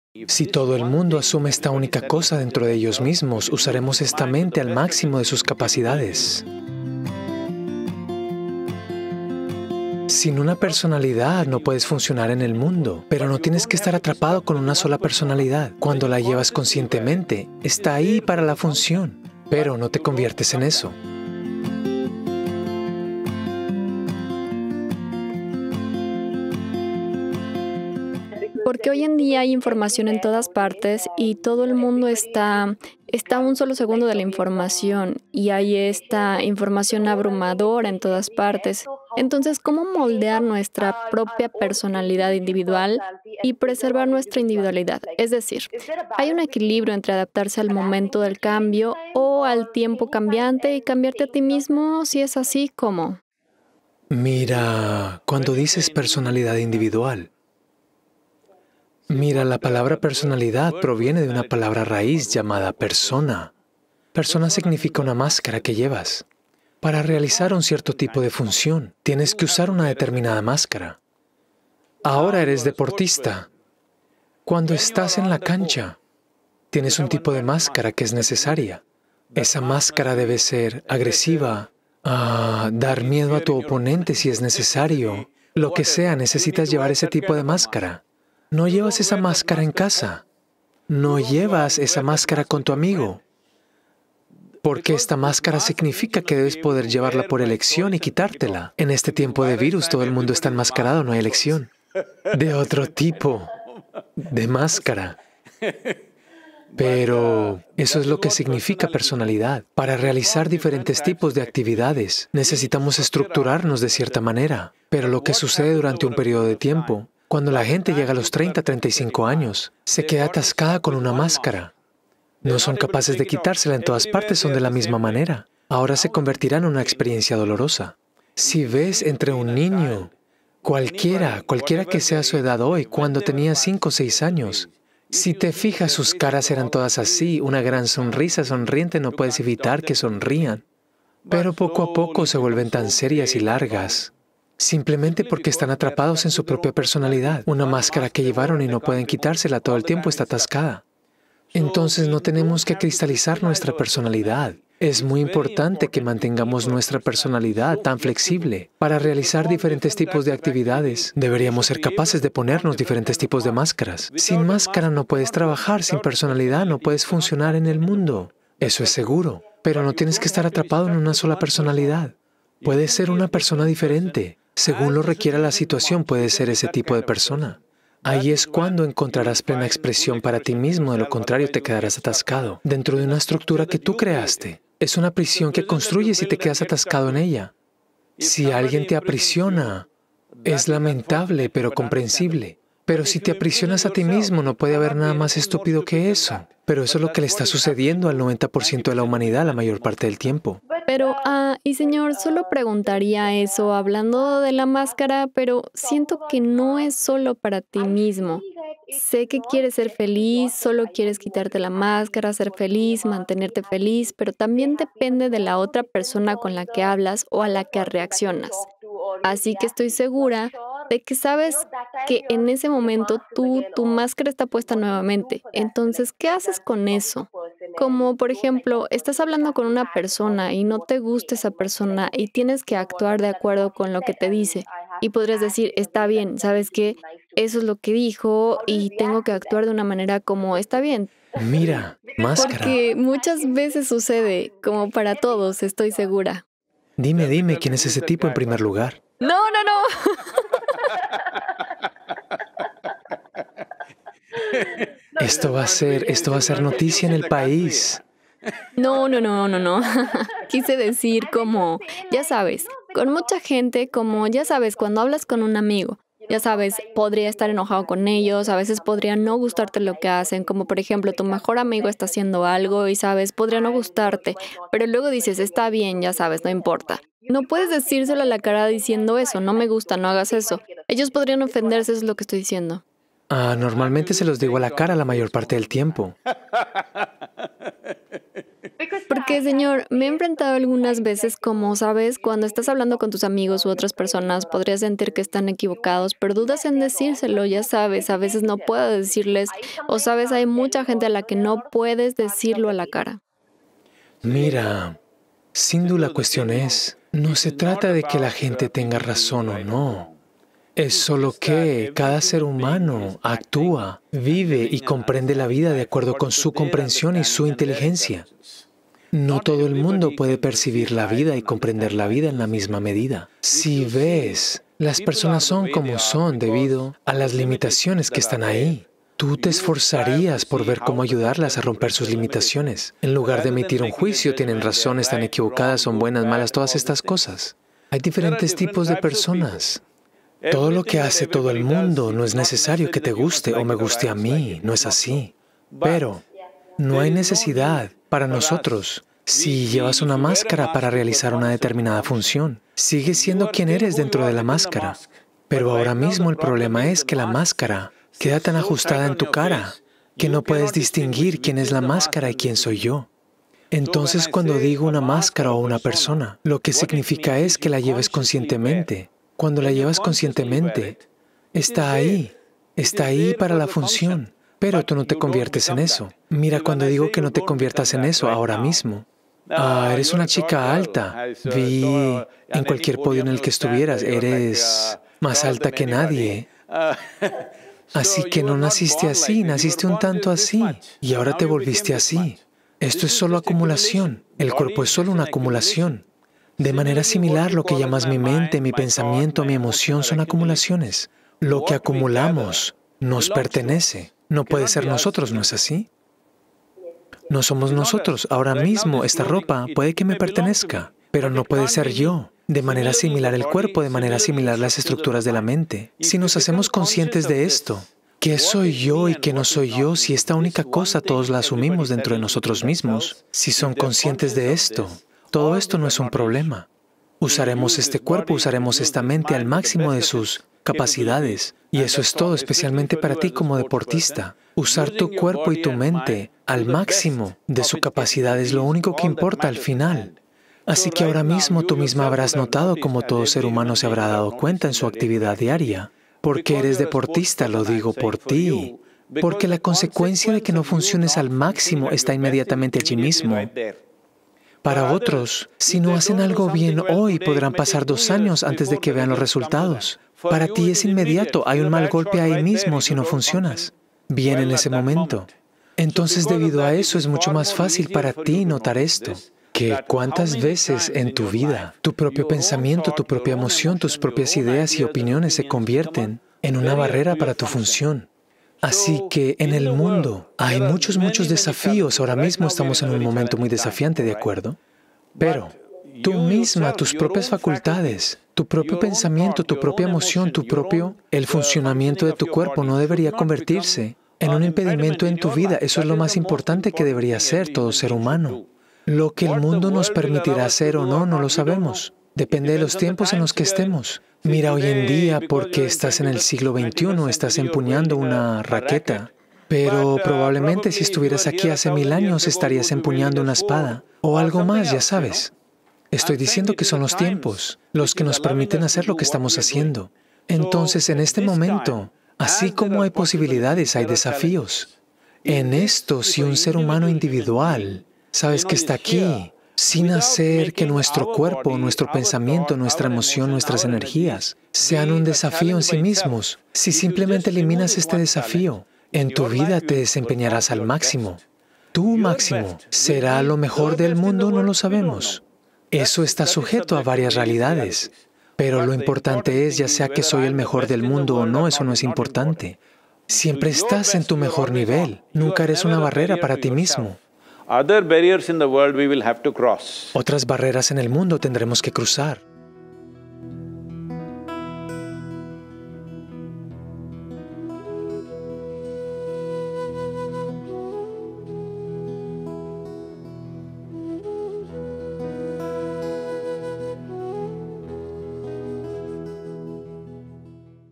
¿Qué tipo de personalidad es mejor? | Sadhguru
Sadhguru observa cómo nosotros mismos creamos nuestra personalidad, y por qué debemos desarrollar la habilidad de poder cambiarla conscientemente según lo requiera cada situación.